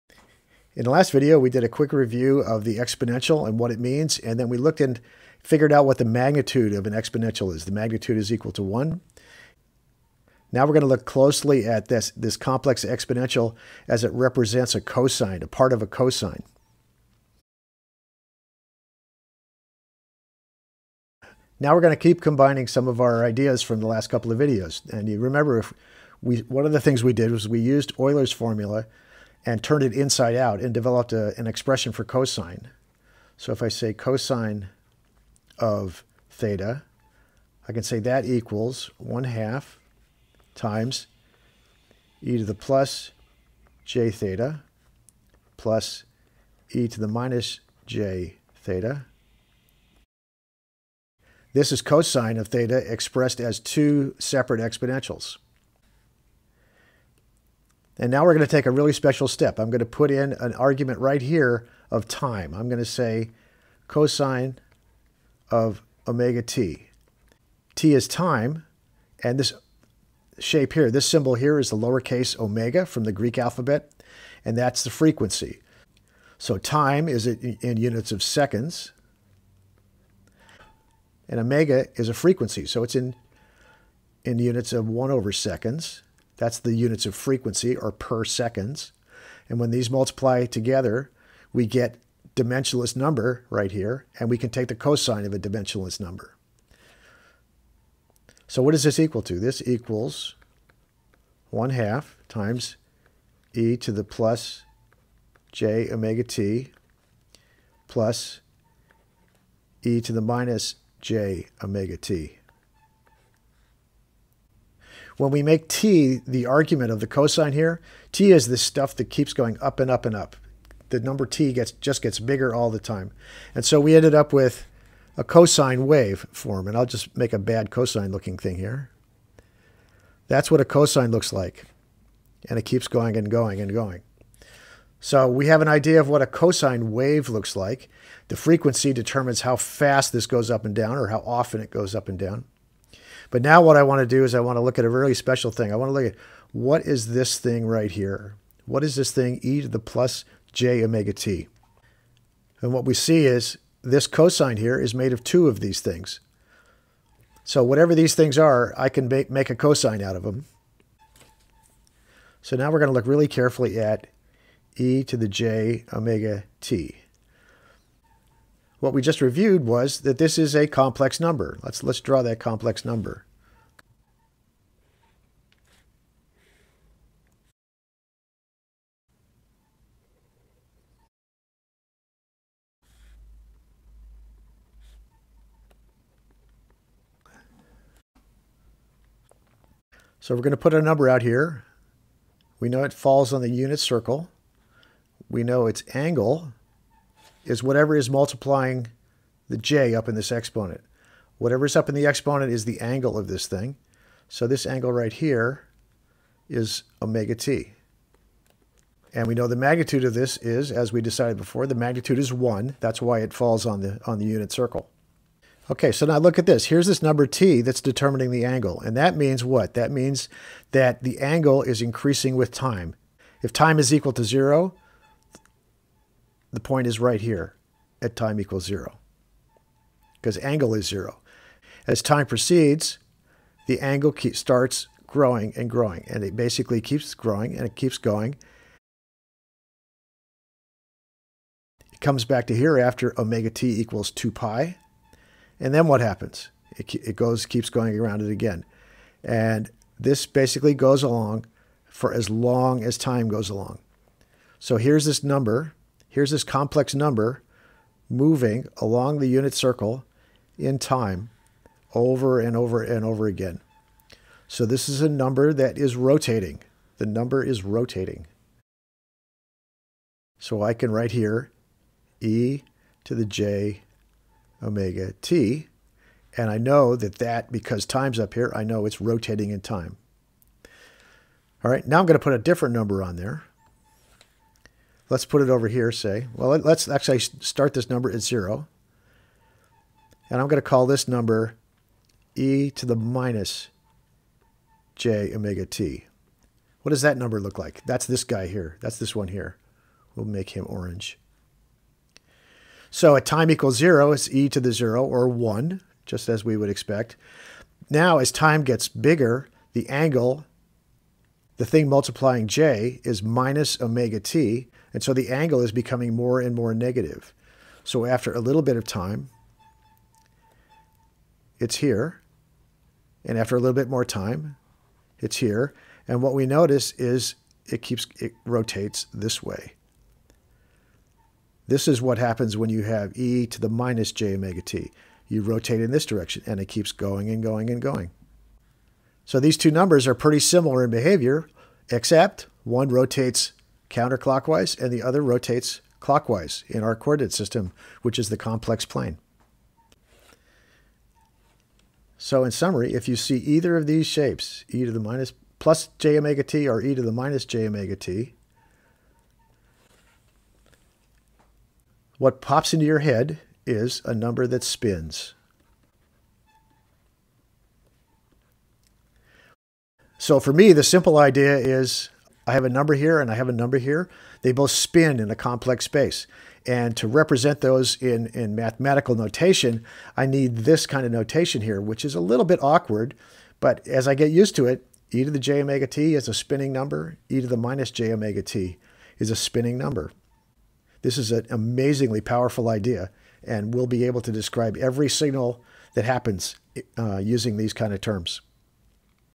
Complex exponentials spin
Courses on Khan Academy are always 100% free. Start practicing—and saving your progress—now

When we put time up in the exponent of a complex exponential, the complex number it represent rotates in a circle on the complex plane. You can think of it as a spinning number!